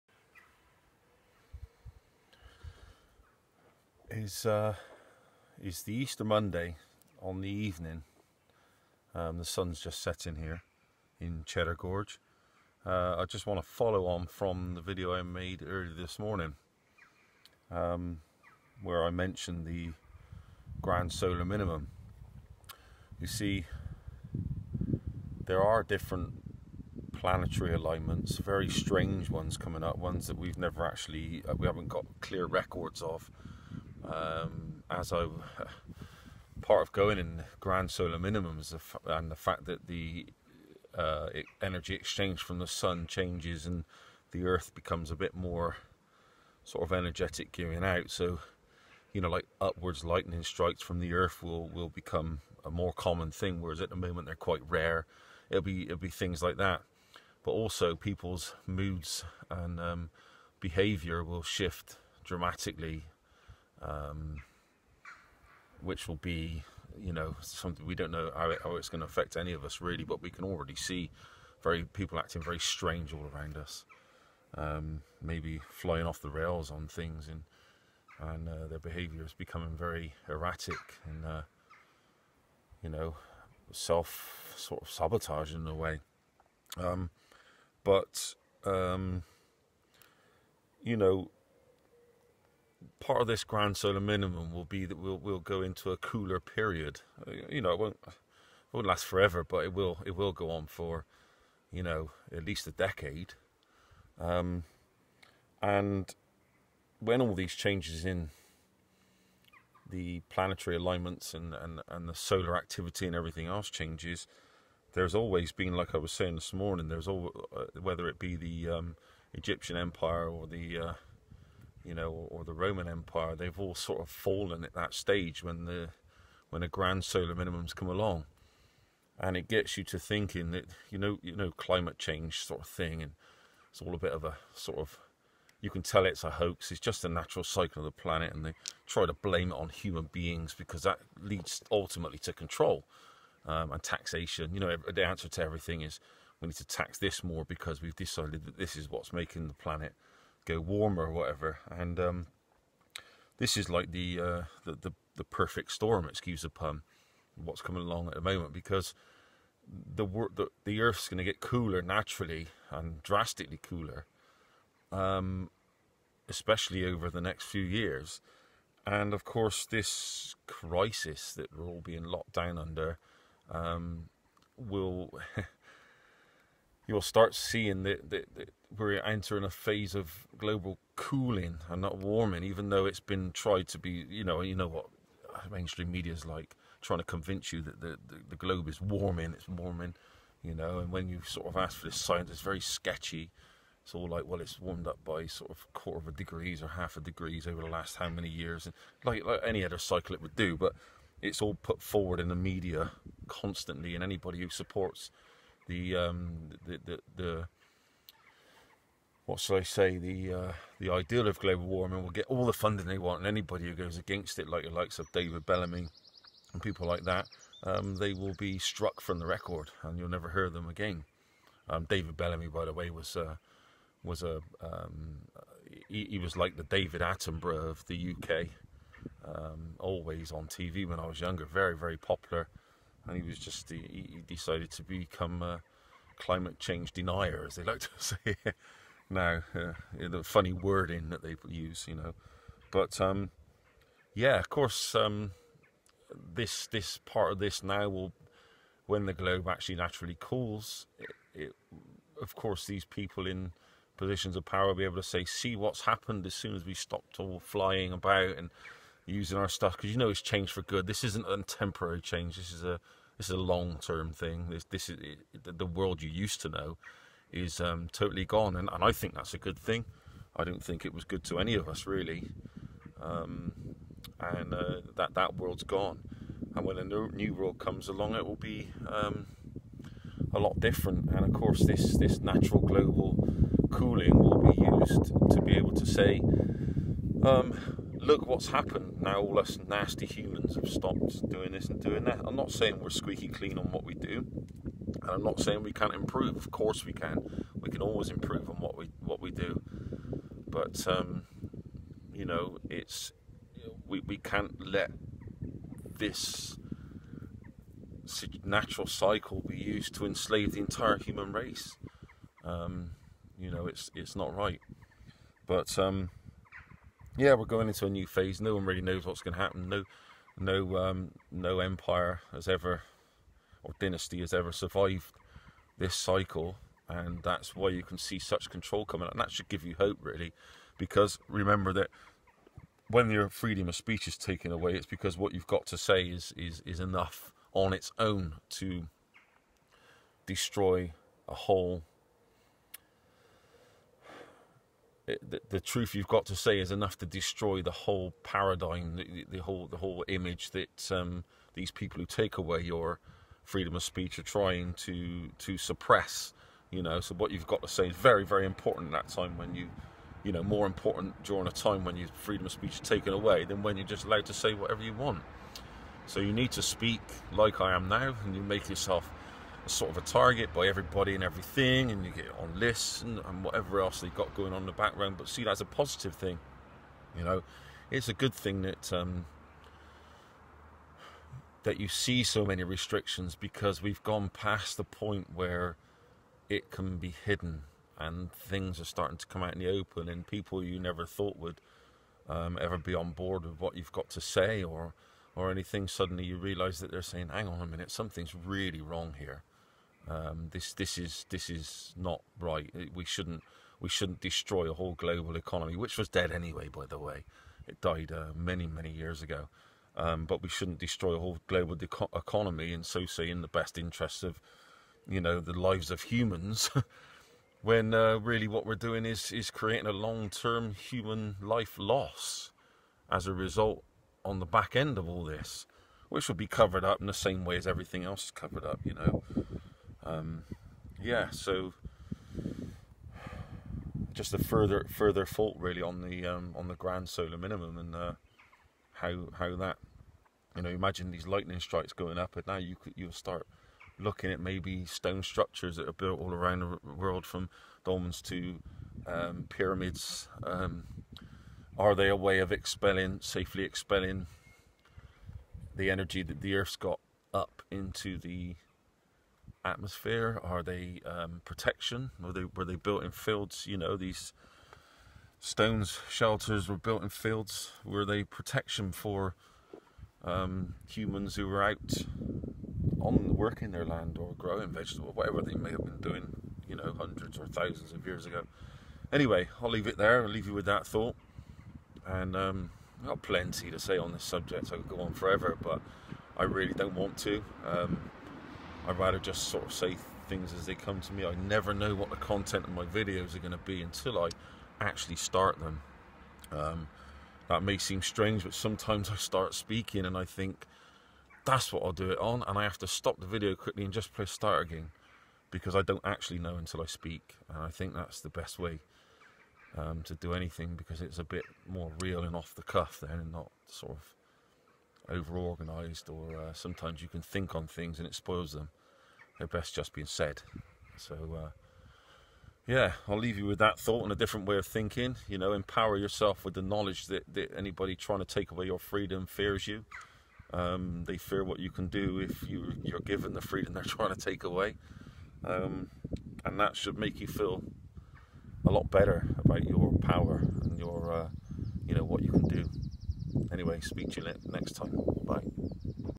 Those who attempt to take your freedom fear you
As we enter a grand solar minimum, fear among people in positions of power starts to heighten, this results in attempted enslavement to try and maintain that power.